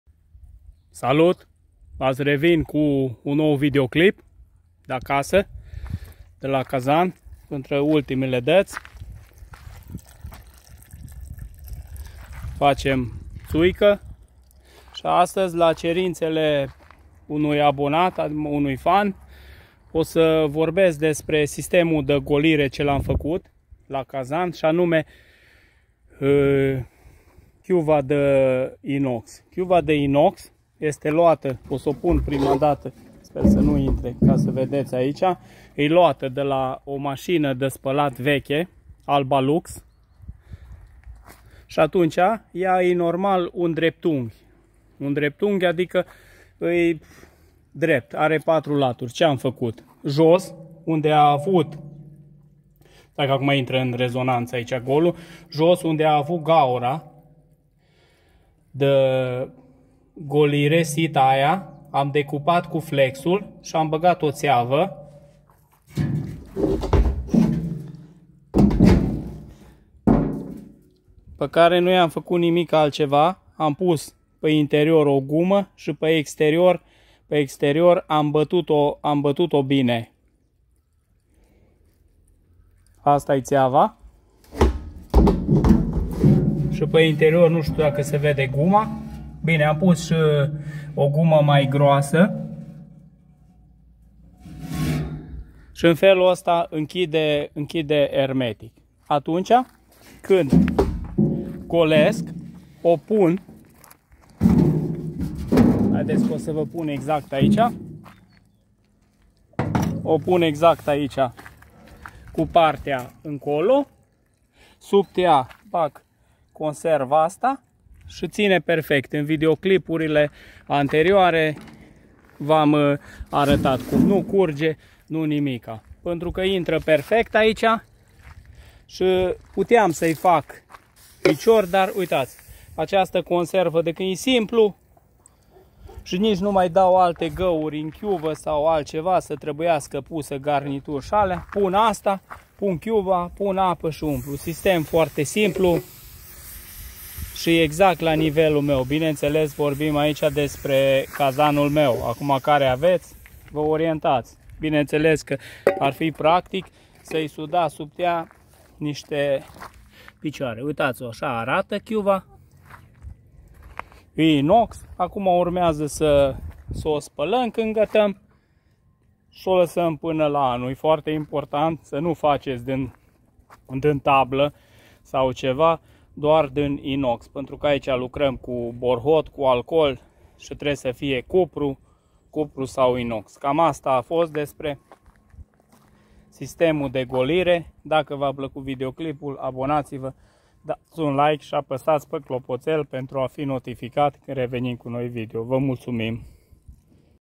Sistem umplere cazan tuica facut acasa
La cererea unui abonat prezint mai in detaliu golirea facuta la cazanul de tuica ,folosim o cuva de la o masina de spalat veche.